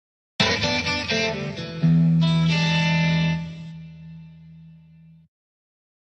My New Dark Gray Intro!
Well this is it! My new dark gray legend intro is here! So, what do you think of my new intro?